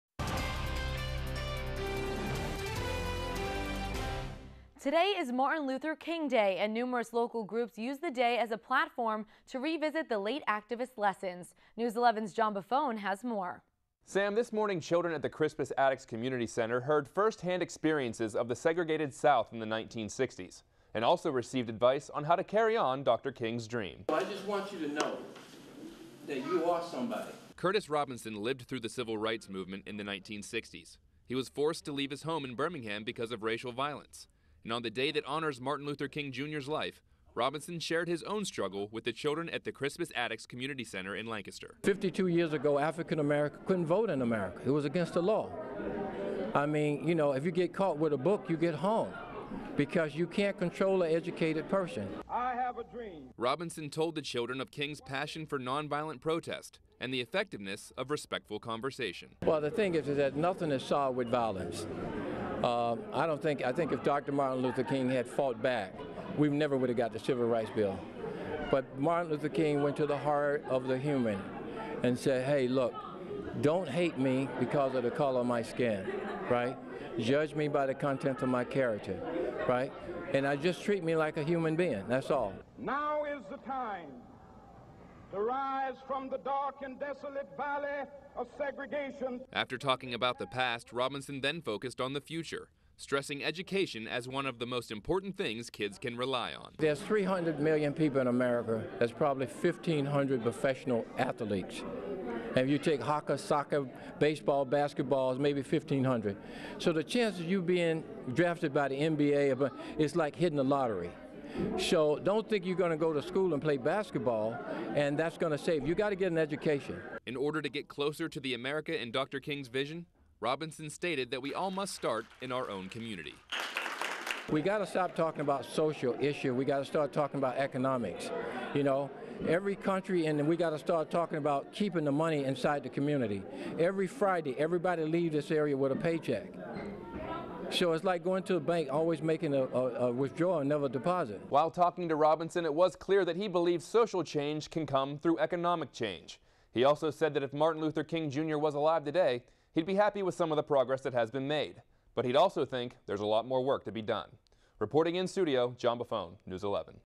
Children at Crispus Attucks learn of MLK's dream
The children at the Crispus Attucks Community Center in Lancaster heard firsthand experiences of the segregated south and Martin Luther King's vision for America.